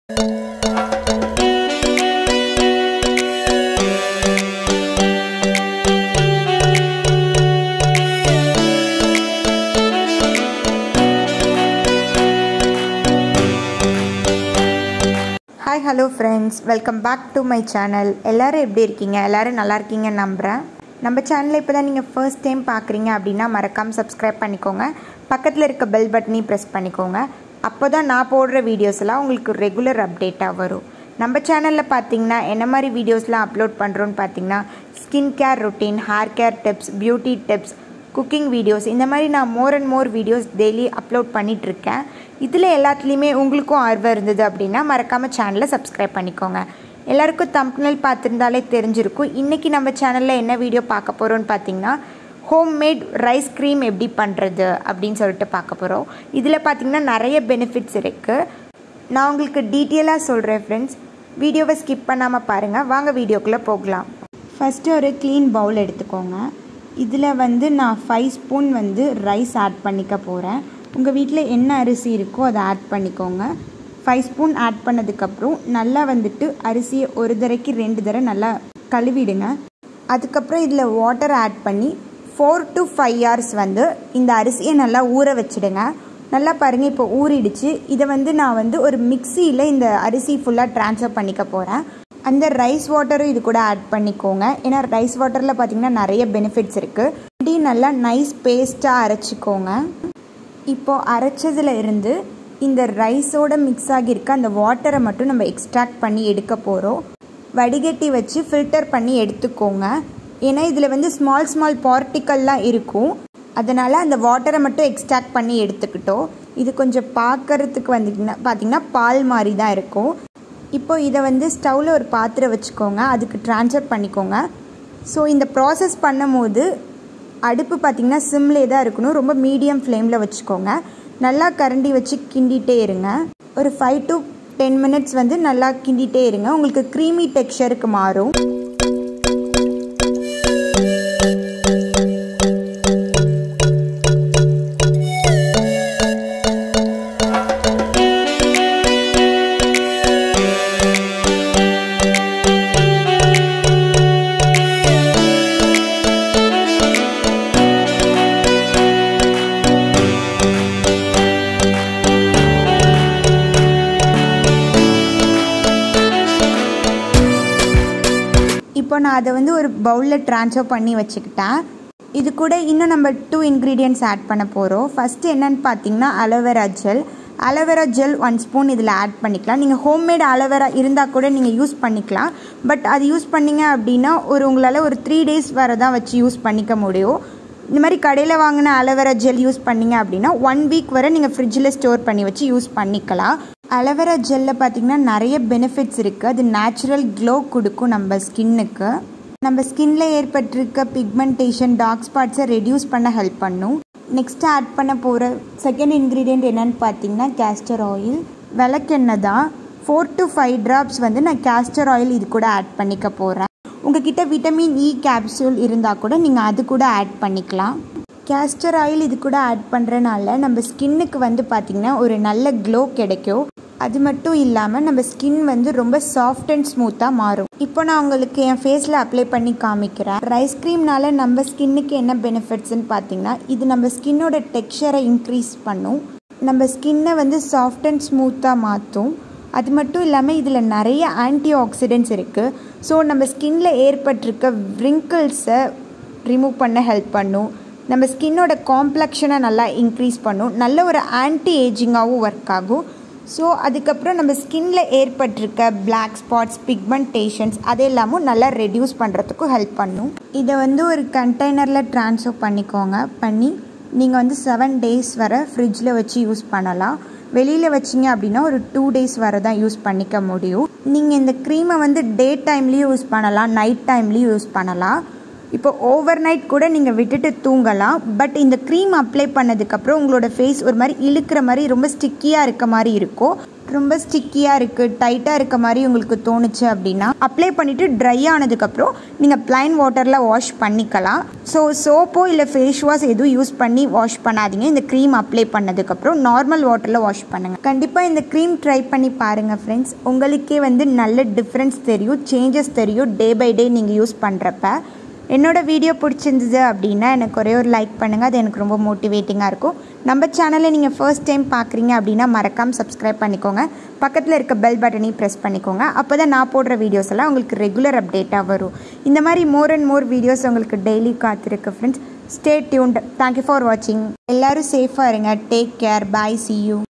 Homemade Rice Cream | in Tamil | How to Make Rice Cream | Get Glowing Skin | Just Try For 7 Days 😍😱
Song Credits
Musician: ASHUTOSH